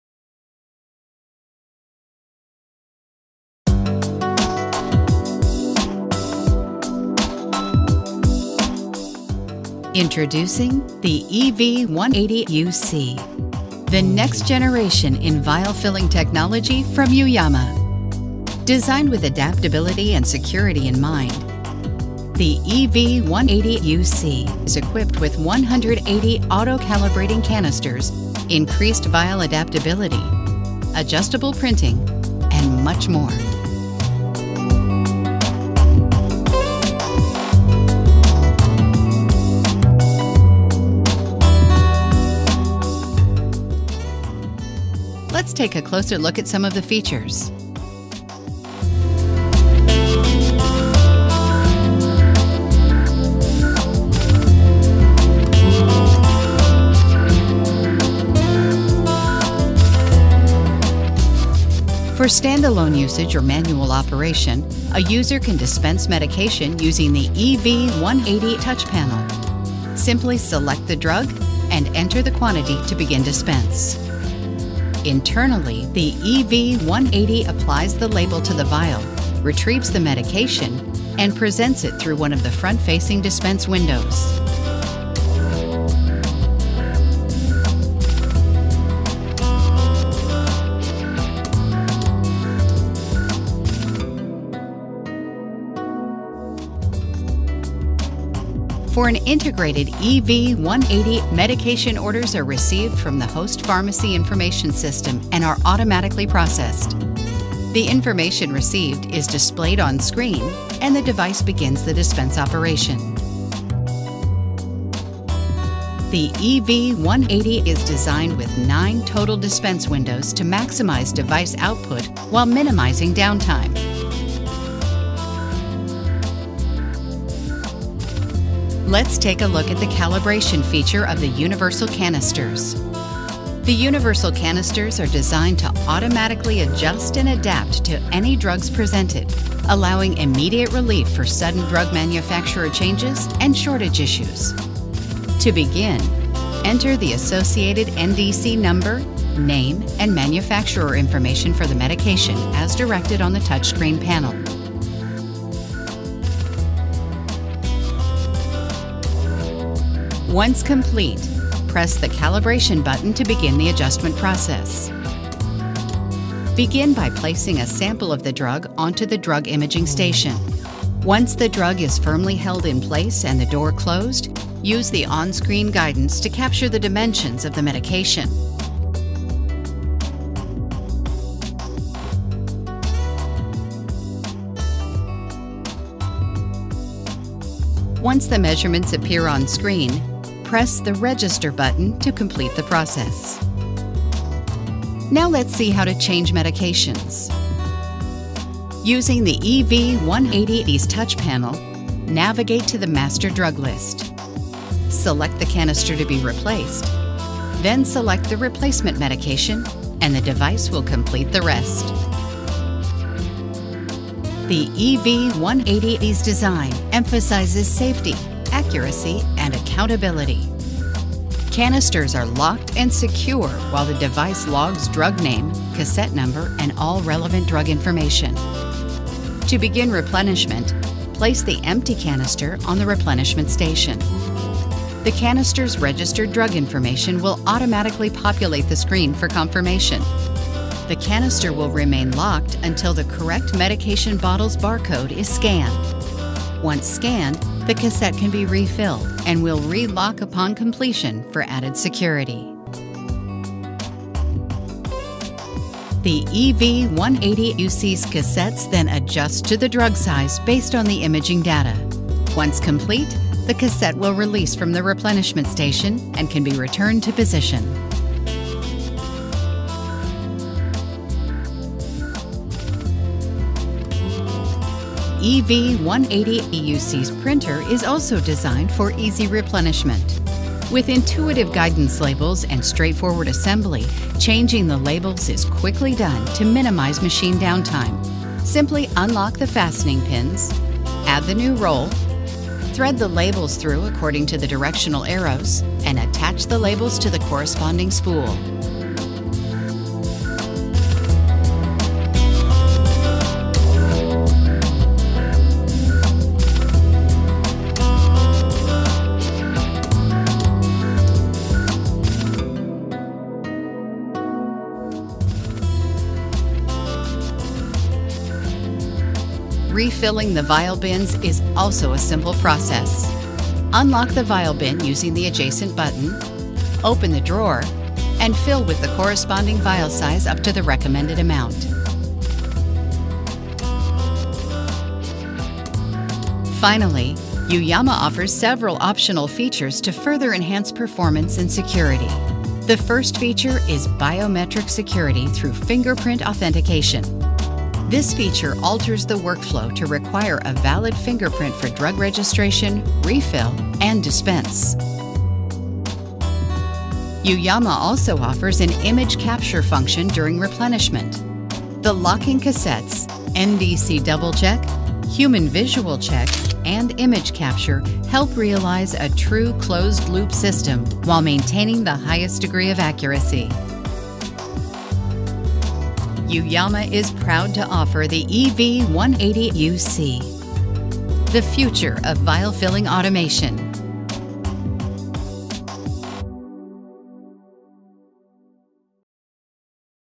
EV180UC Fully-Automatic Vial-Filling Machine
The EV-180UC "Universal" is the newest generation of vial-filling robotics from Yuyama.EV-180UC fills 50% daily script volume or more, giving pharmacy staff more time to ensure customer satisfaction.